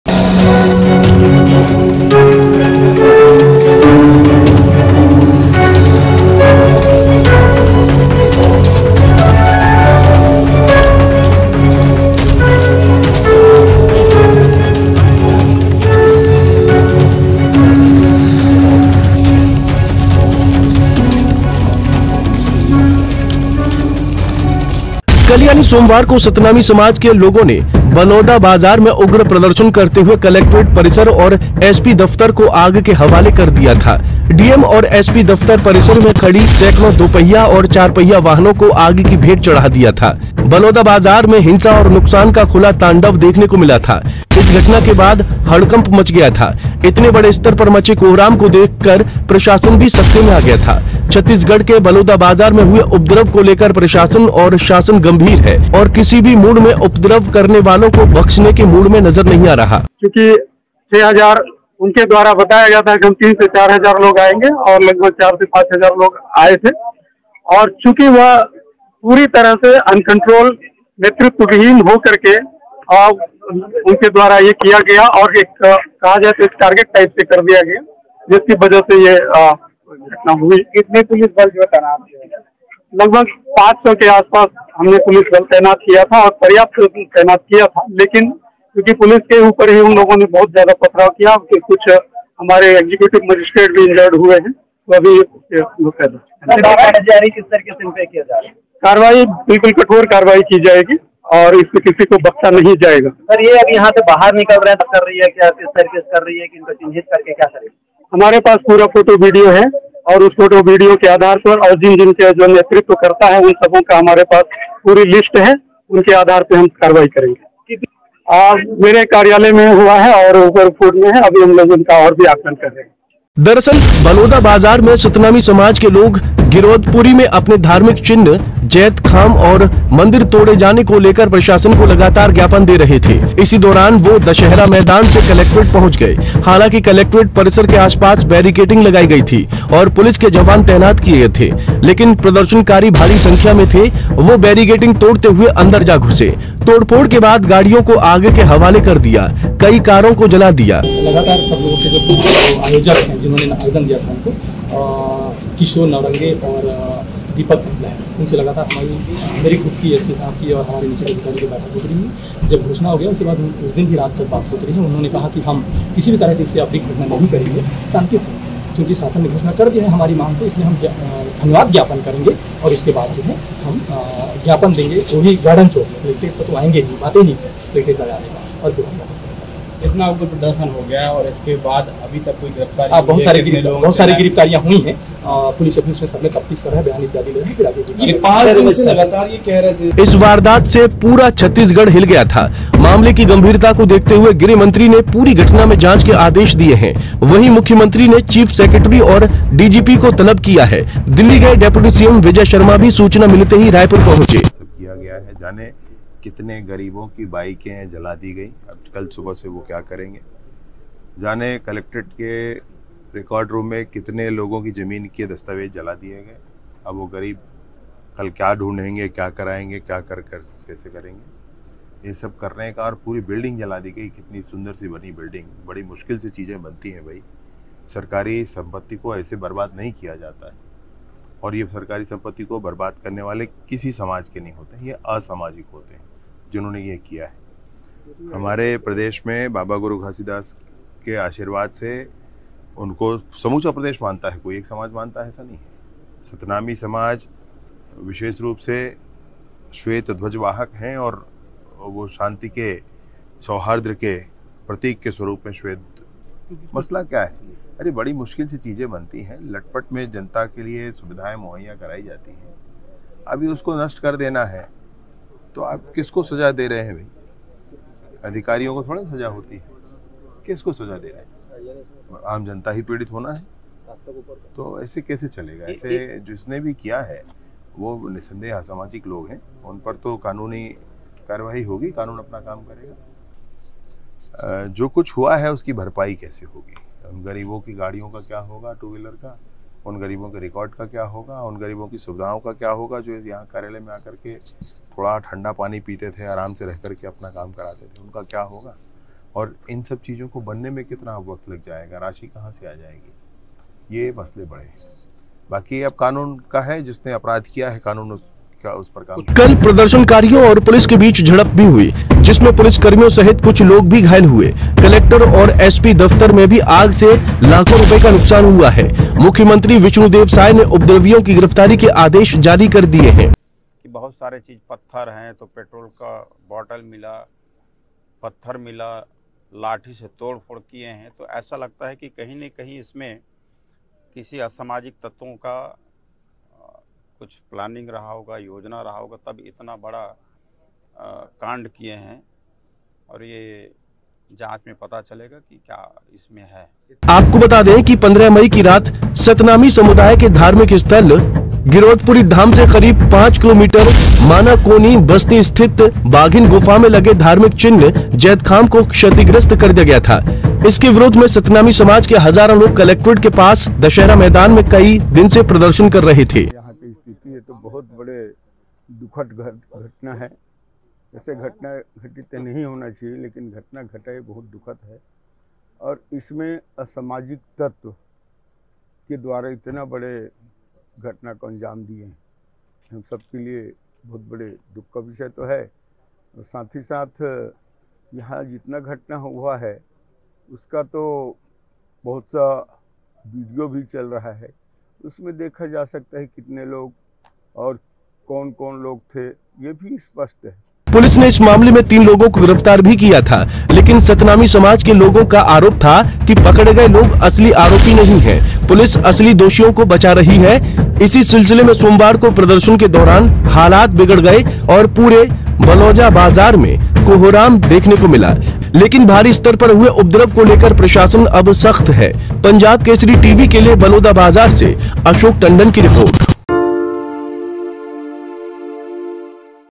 बलौदाबाजार हिंसा पर गंभीर सरकार! उपद्रवियों पर होगी कठोर कार्रवाई! प्रशासन बख्शने के मूड में नहीं !
Also Watch ►